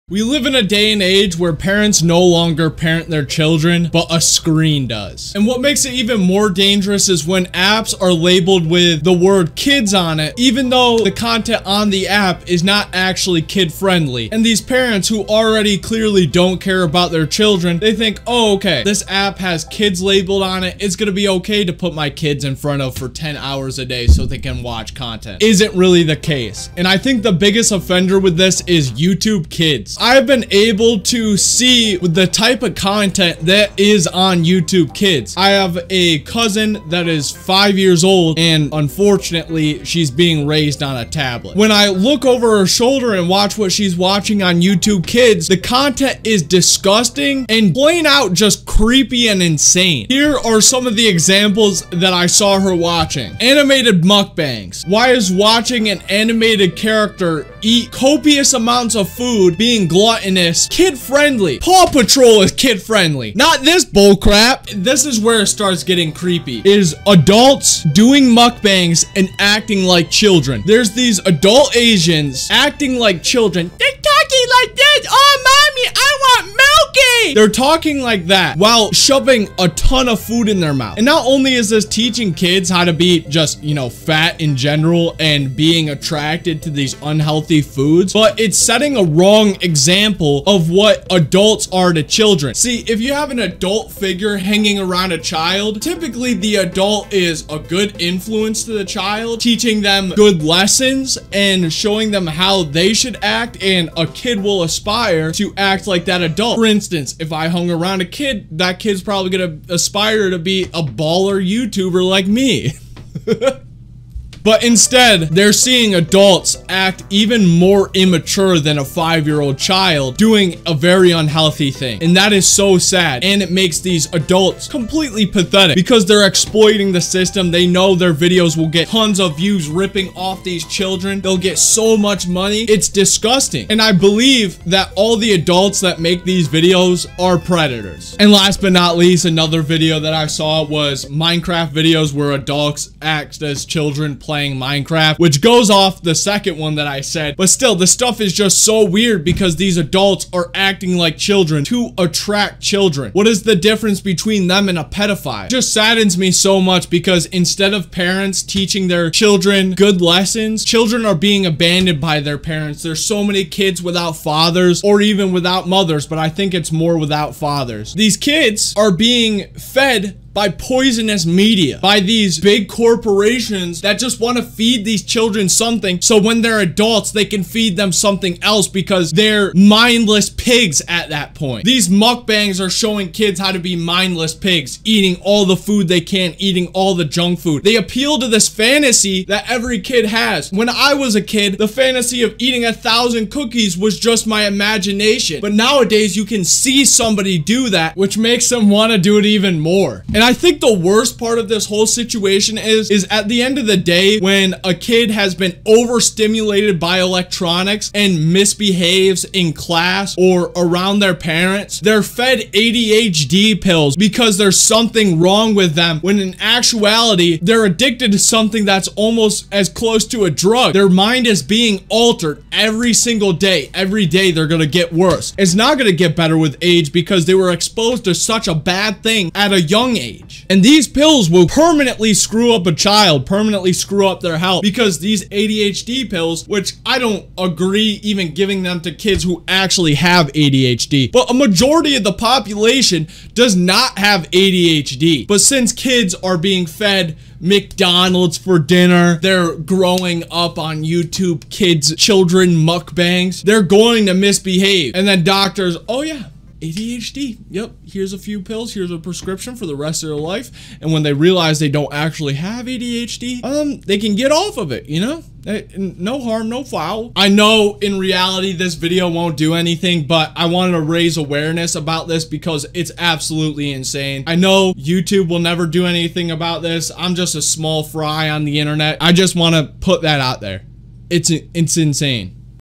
Is YouTube Kids Making Children Stupid?
In the day and age where children are no longer raised by a parent, but by a screen. I ask the question is the app YouTube Kids making children stupid? Its not just stupidity, but its many other traits aswell. YouTube kids is a platform where it supposedly has "Child friendly" Content when if you actually watch the videos youd find that isnt the case.

~ Nerdperior ♡